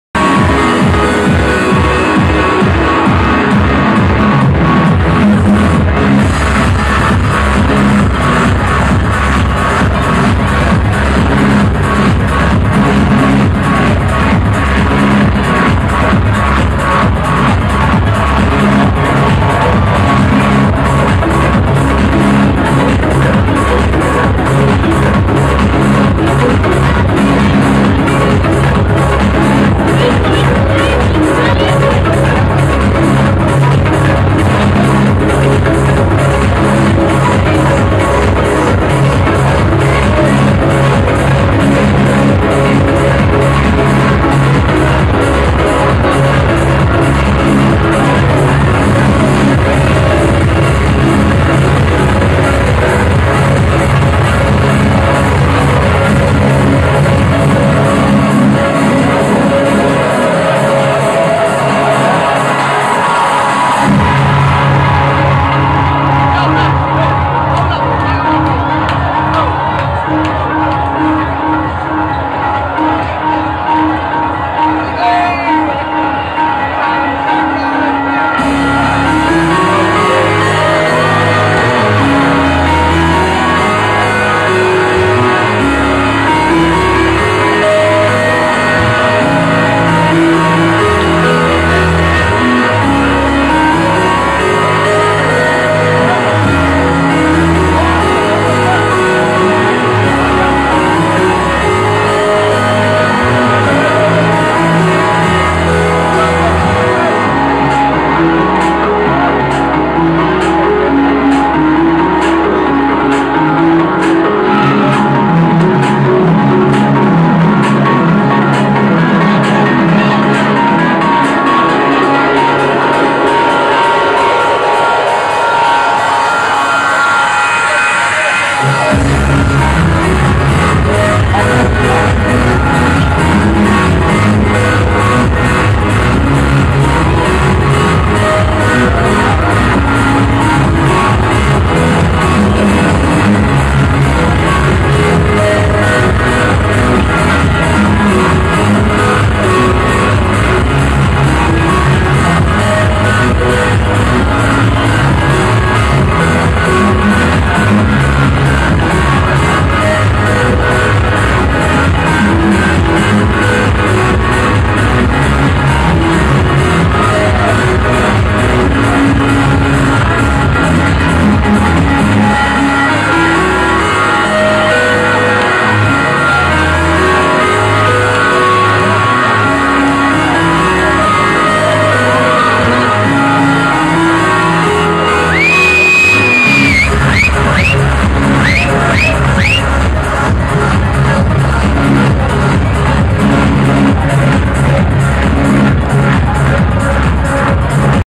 Markus Schossow digital goodgreef 290110
Hello had another fantastic night on friday night at digital for mEddie Halliwell and Goodgreef. Big shout to Braddas for driving. Hope you enjoy the vids. Mucho Gracias amigo's!